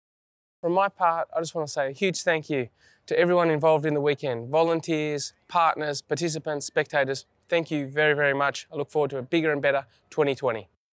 Cadel's Thank You Message
Cadel Evans says thanks for making the 2019 edition of the Cadel Evans Great Ocean Road Race the best yet.